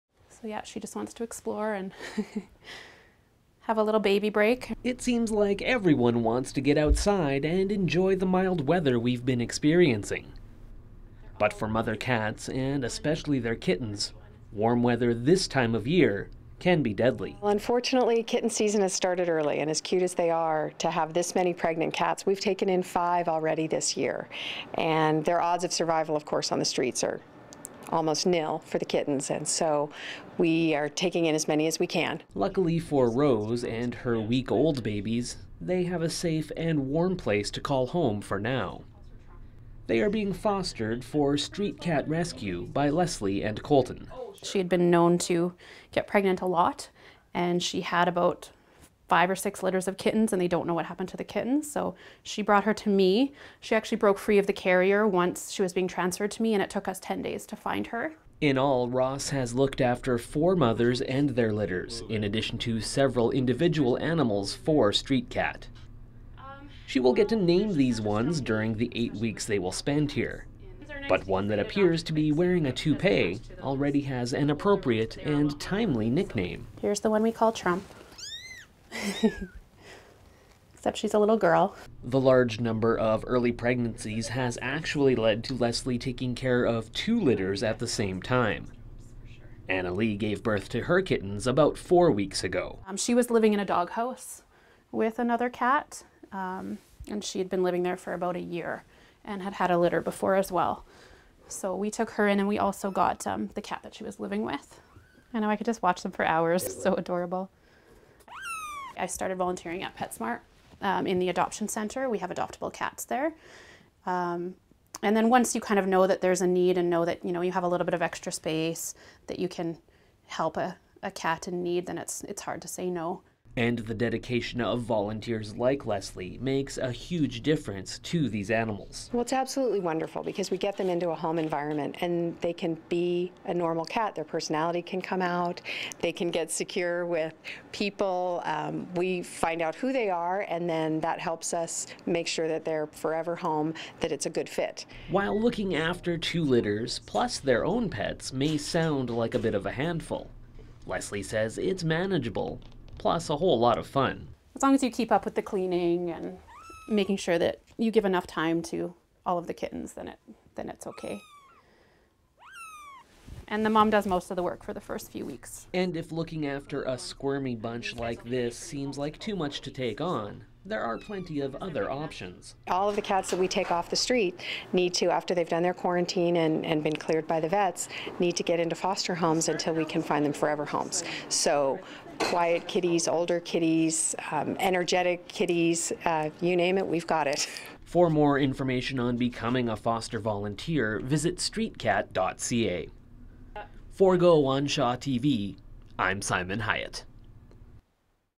SCAT Early Kitten Season
The unusually warm weather isn’t all good news as there has been a spike in pregnant cats in the city and their kittens could be at risk.